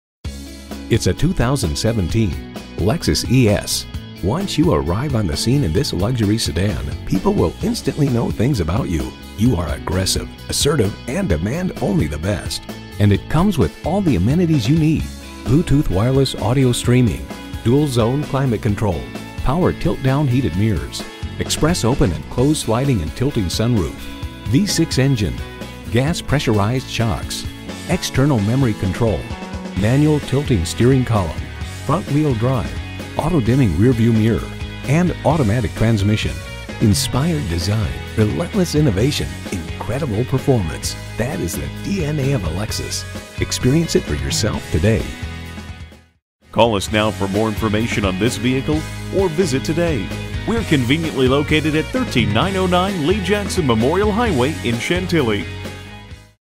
Used 2017 Lexus ES Chantilly Dale City VA DC, MD LP180263 - SOLD
Year: 2017
Make: Lexus
Model: ES
Trim: ES 350 FWD
Engine: 214 L V6 Cyl.
Transmission: Automatic
Color: Satin Cashmere Metallic
Interior: PARCH
Mileage: 8957
Stock #: LP180263
VIN: 58ABK1GG3HU062481

Address:
13909 Lee Jackson Memorial Hwy